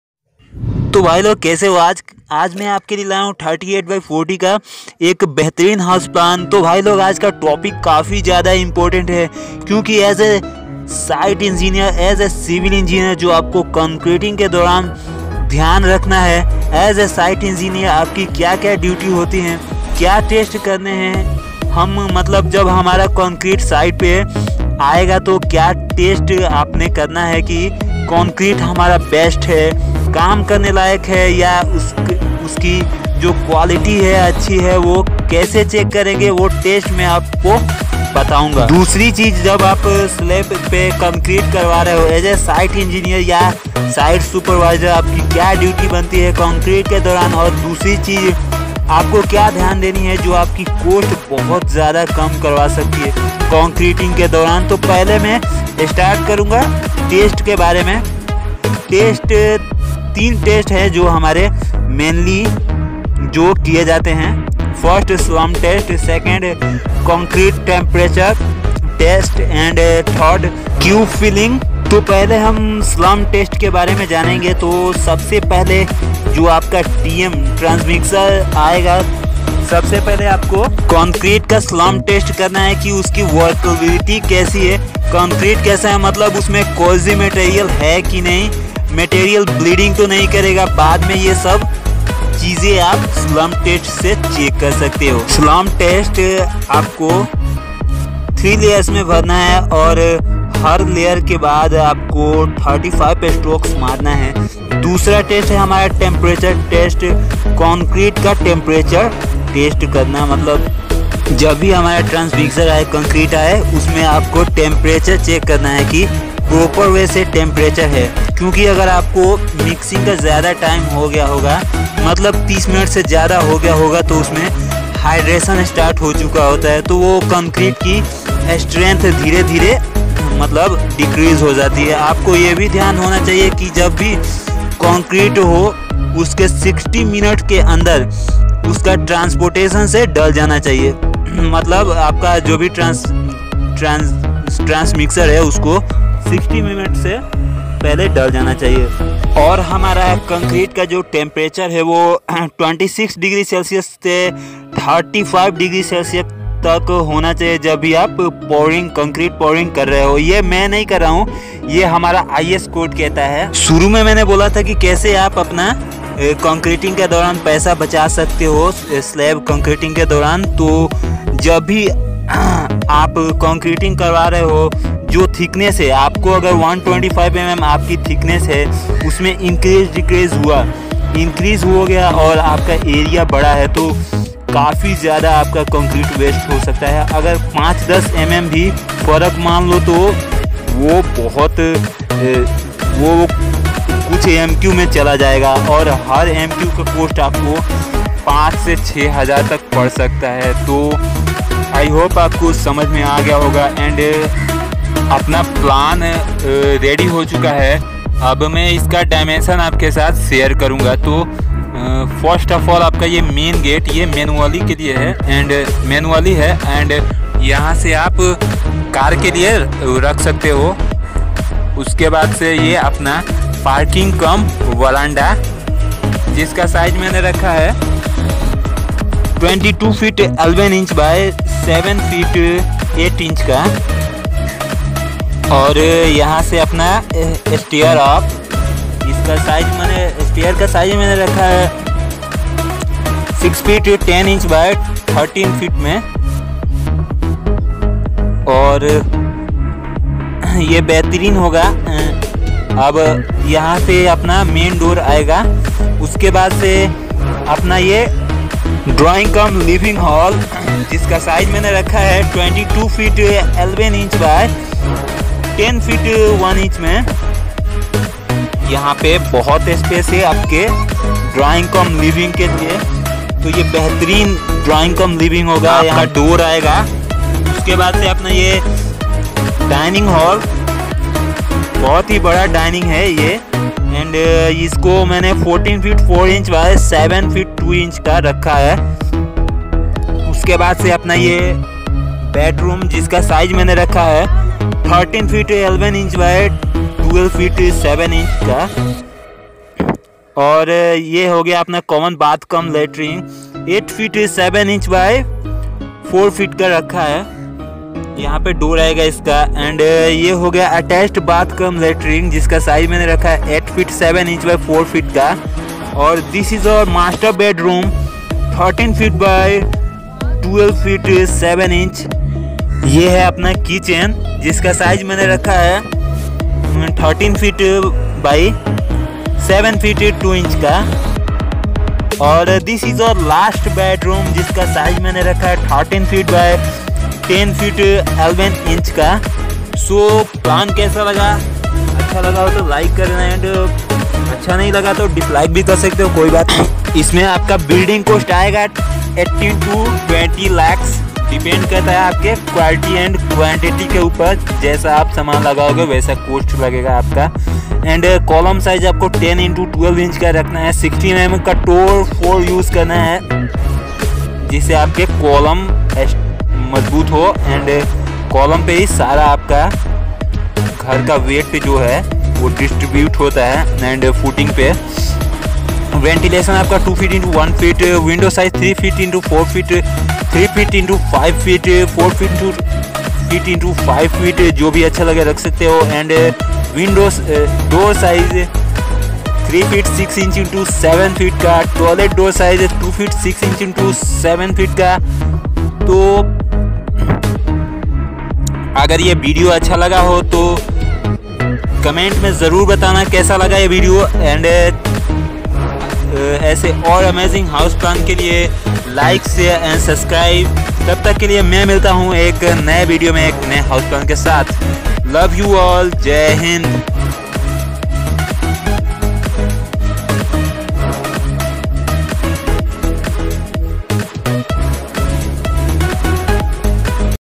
38×40 house plan// 1520 sqft house design//Responsibility of civil engineer
38×40 house plan
38×40 house design
1520 sqft house design
Responsibility of civil engineer
House plan with vastu

इस विडियो में मैंने एक 38×40 घर का नक्शा डिजाइन किया है
तो विडियो अच्छा लगा हो तो कृपया

And if you guys have not followed me yet